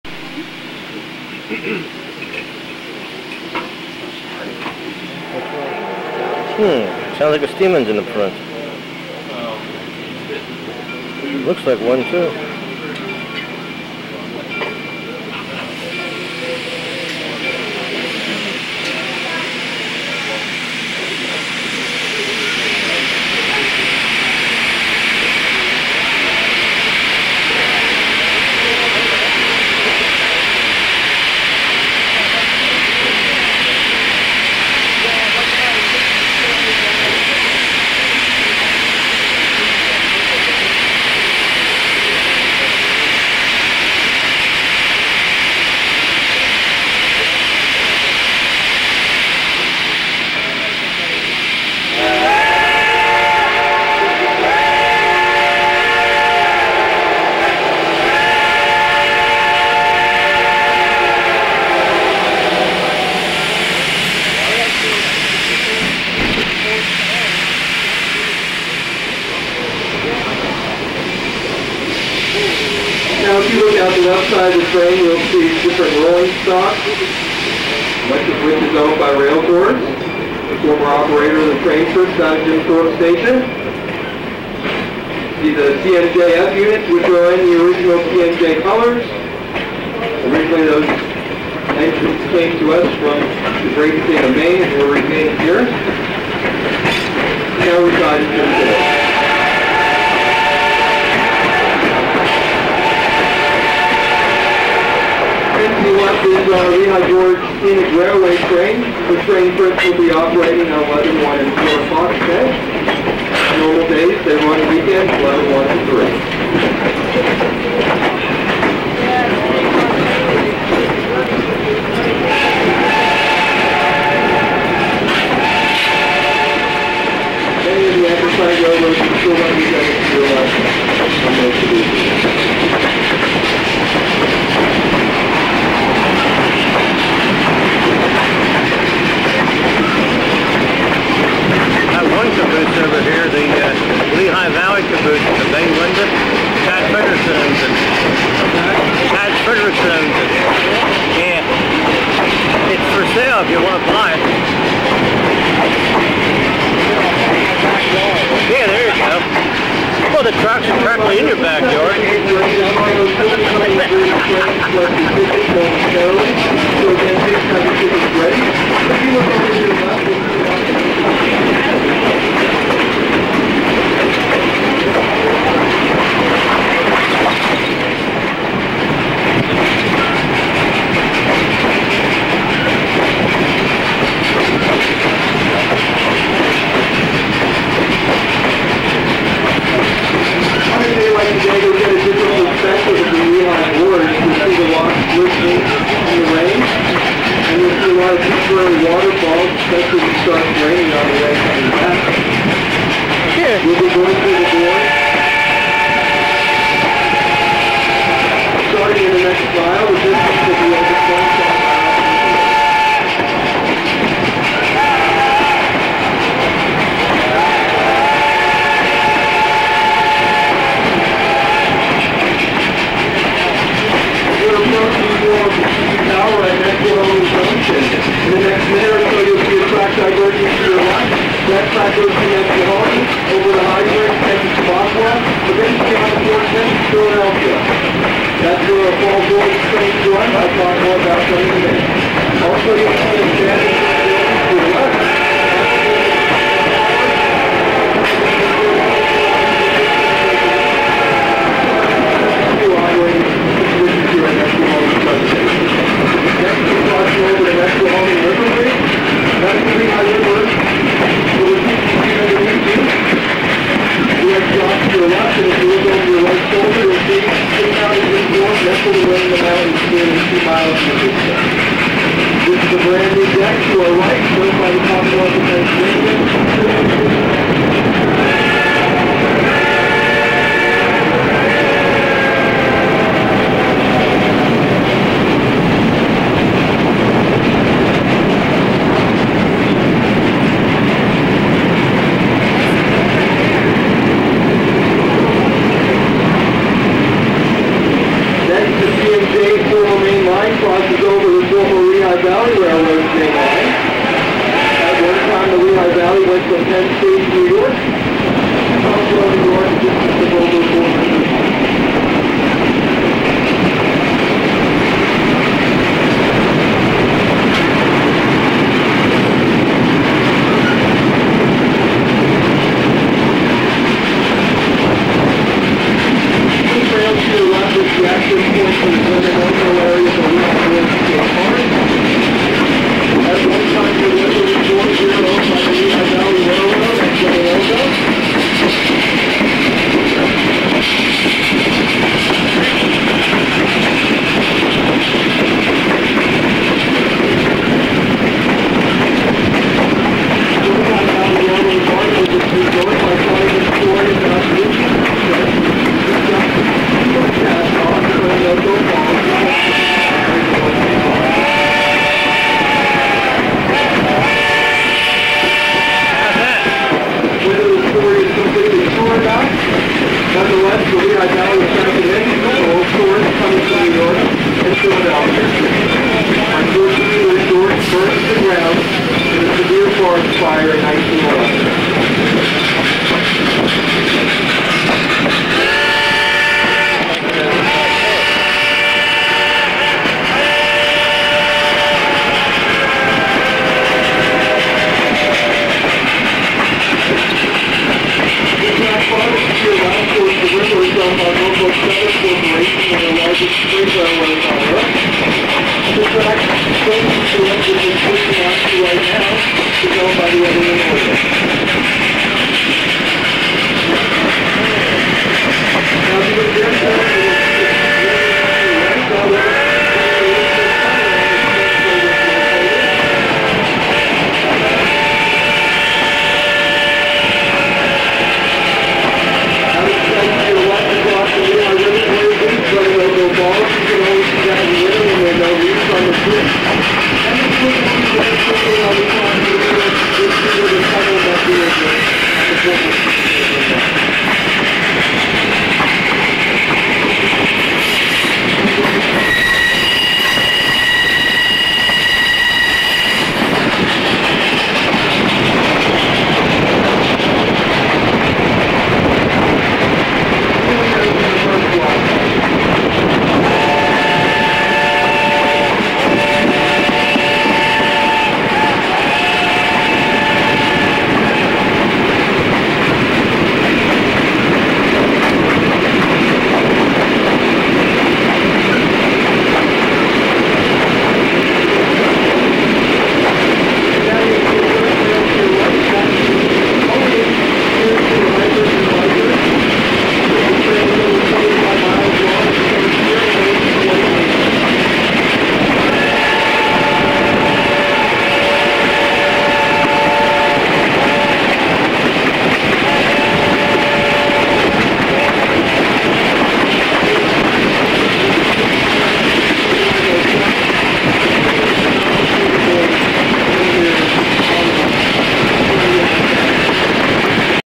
Reading and Northern 425 Riding Behind BLUE THUNDER Part One
Our journey takes us to Jim Thorpe, PA. where we will be riding behind Reading Blue Mountain & Reading's steam locomotive No. 425. Our trip takes us to Pittston, PA. and return to Jim Thorpe.
 This trip was a bit different because of Tropical Storm Hanna,  that was coming up the east cost, But 425 and the help of an SD-50 No. 5049 pulled the train with not to much problems, With wet rails,   425 put on a great show of grace and power. proving to the world that "Steam Is Still King !".
 Our Train will be operating over one of the oldest routes in the county. The late Asa Packer's Lehigh Valley Railroad first laid the tracks around Coal Town now known as Jim Thorpe in the 1850's. By 1864, the Lehigh Valley Railroad had extended its trackage north to White Haven. By 1868, the tracks was then extended north through Pittston.
 So sit back an enjoy, your journey back in time. All Aboard!